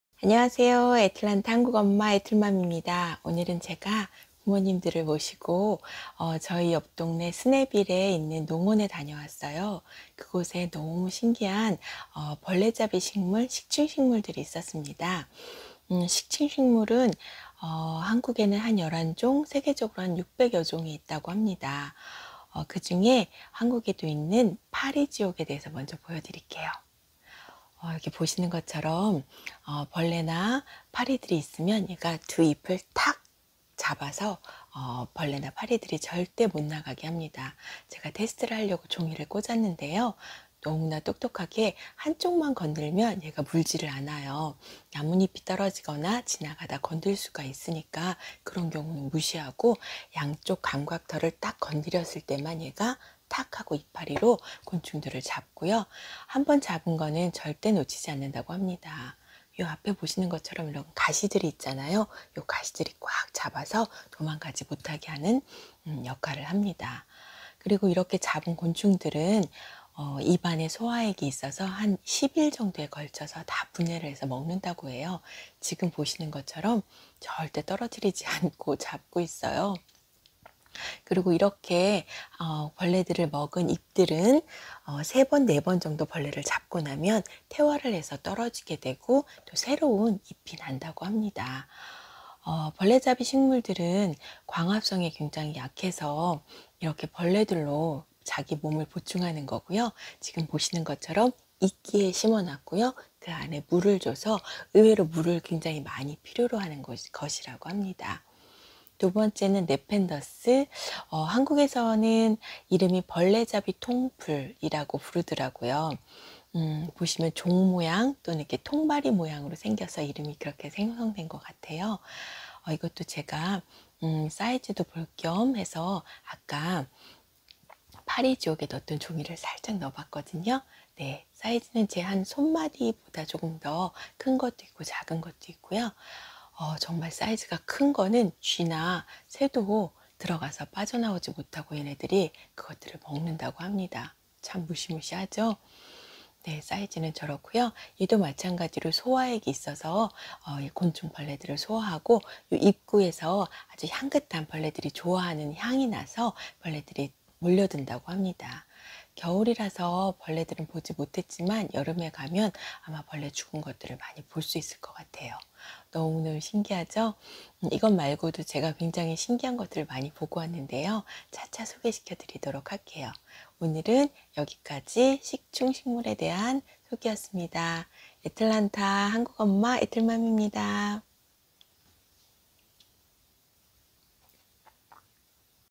[미국일상VLOG]벌레잡이 식물(식충식물) 파리지옥과 벌레잡이통풀, (Venus Flytrap, Tropical Pitcher Plants)
부모님과 스네빌에 있는 농원에 다녀왔습니다.
신기한 식물들이 많았어요.
벌레잡이 식물(식충식물)을 소개해 드릴께요.
(특징)
1) 벌레잡이 식물은 광합성을 잘 못해서 벌레로 몸에 필요한것들을 보충합니다.
2) 물을 많이 필요로 해서 이끼가 많은 곳이나 물이 많은곳에서 자라고 길러야 한다고 합니다
3) 벌레,곤충 유인은 소화액에 아주 좋은 향이 나와서 유인한다고 합니다.

1.파리지옥 (Venus Flytrap)
1) 포획형
2) 감각털을 양쪽 또는 두번 건들면 1초안에 벌레를 잡는다.
3) 뽀족한 가시가 입구를 막아서 절대로 곤충들이 나갈수 없다.
4) 입안에 소화액이 곤충을 녹여서 10일안에 다 녹여 먹음
5) 3~4번 벌레를 잡아 먹으면 퇴화해서 입이 떨어진다.

2.벌레잡이통풀=네펜데스(Tropical Pitcher Plants)
1) 통입구에 꿀샘이 있어서 벌레들이 먹으러 왔다가 미끄러지고 안에 소화액에 녹아 없어지게 된다.
2) 큰 네펜데스는 쥐나 새도 먹을수 있다.

(위치 : The Family Tree Garden Center)
1983 Main St E, Snellville, GA 30078